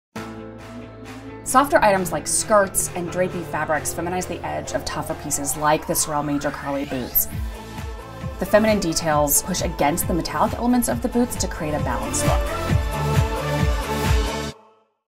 SORELstyle by Jenn Rogien: Mixing Elegant Pieces with Tough Boots
SORELstyle is a video series designed to bring you inspiration from style influencers around the globe. Featuring the new Fall 2015 style, the Major Carly, costume designer Jenn Rogien dresses Emma in a mix of artistic and versatile pieces to capture her love of music and classic style.